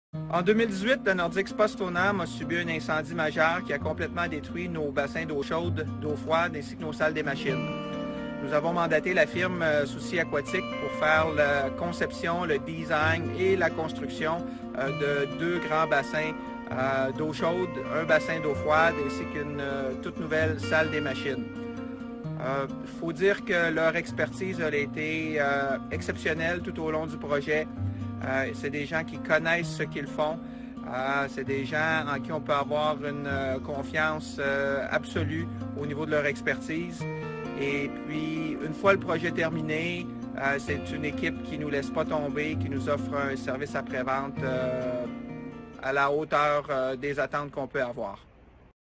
Nordic Spa Stoneham testimonial
We had the pleasure of working with Hugues on his Nordic Spa project.  Thank you for putting your trust in Soucy Aquatik!